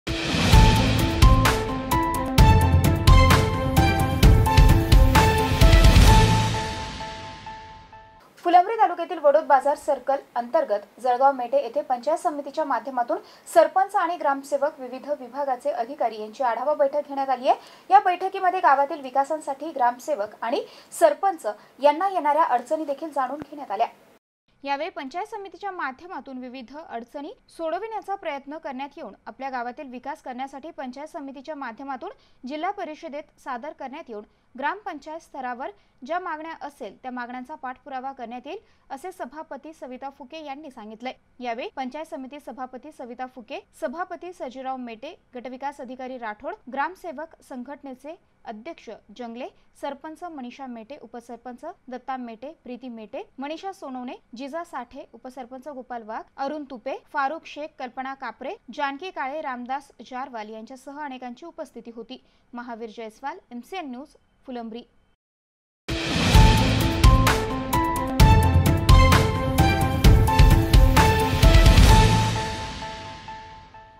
MCN NEWS | पंचायत समितीच्या माध्यमातून सरपंच आणि ग्रामसेवक विविध विभागाचे अधिकारी यांची आढावा बैठक
NEWS UPDATED
NEWS UPDATEDS TODAY
MARATHIYOUTBE NEWS UPDATES
UPDATED NEWS

फुलंब्री तालुक्यातील वडोद बाजार सर्कल अंतर्गत जळगाव मेटे  येथे पंचायत समितीच्या माध्यमातून  सरपंच आणि ग्रामसेवक विविध विभागाचे अधिकारी यांची आढावा बैठक घेण्यात आली या बैठकीत गावातील विकासासाठी ग्रामसेवक आणि सरपंच यांना येणाऱ्या अडचणी जाणून घेण्यात  आल्या